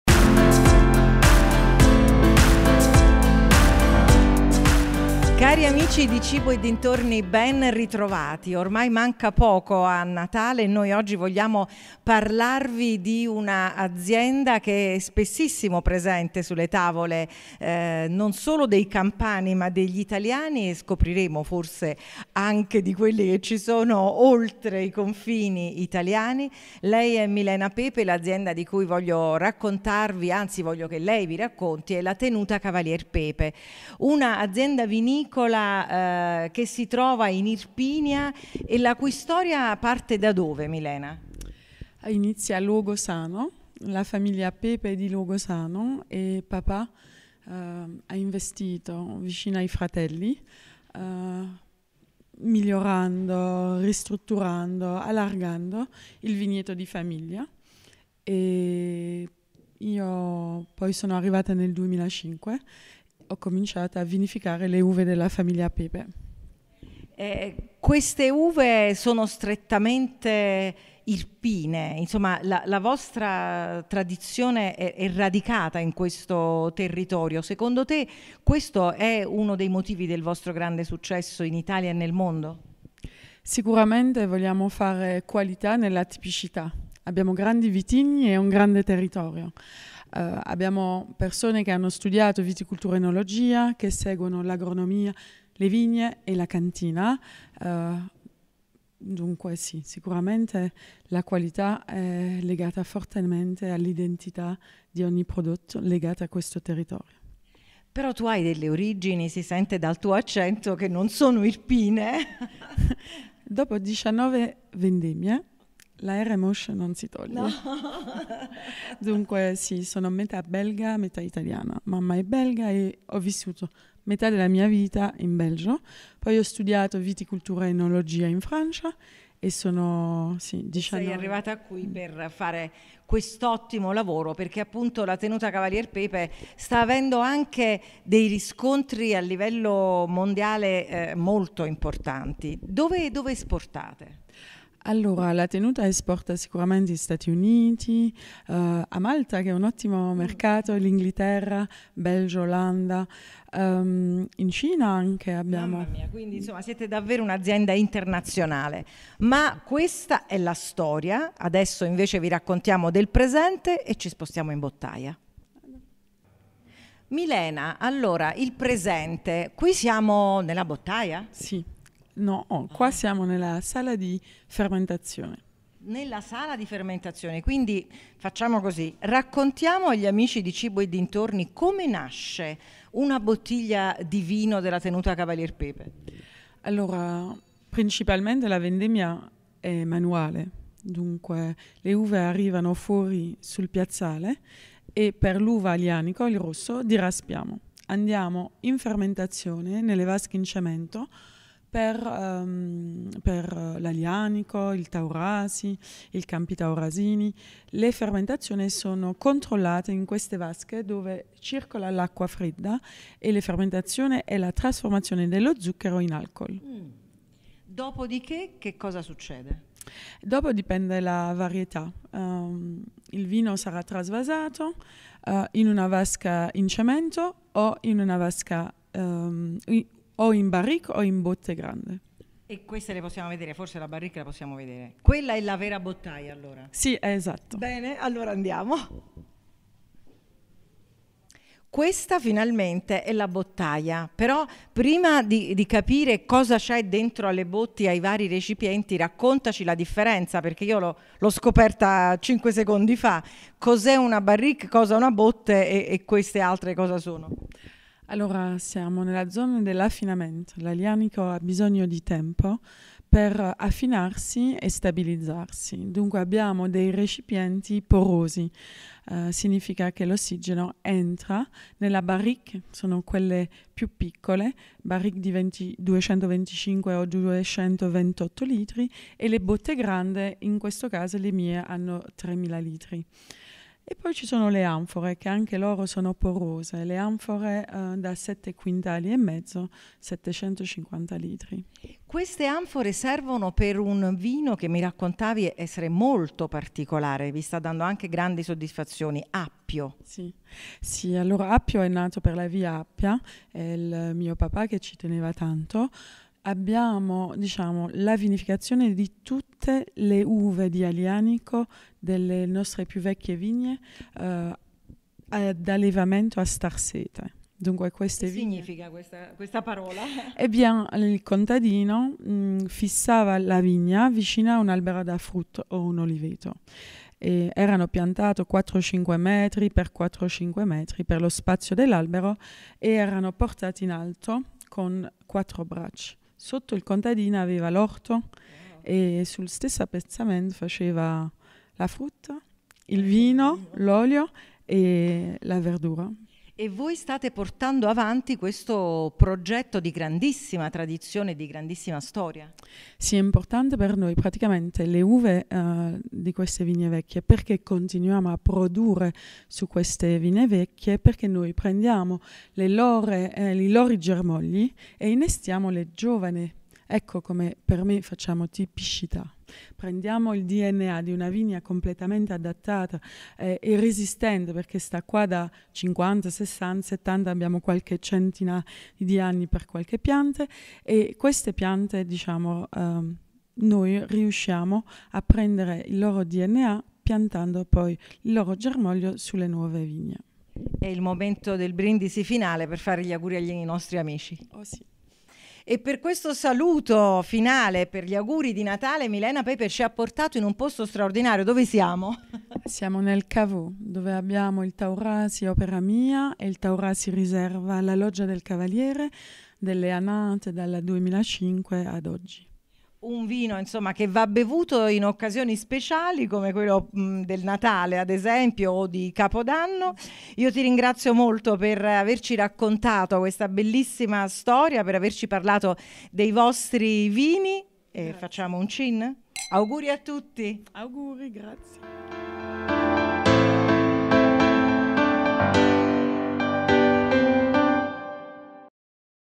Tenuta Cavalier Pepe la storia di un'Azienda vinicola
A raccontarcela è Milena Pepe, col suo inconfondibile accento! Quella della Tenuta Cavalier Pepe è una storia di amore per la terra e per il vino, per la qualità e per la continua scoperta di tradizioni che possono essere perpetrate solo attraverso la grande passione che anima la famiglia Pepe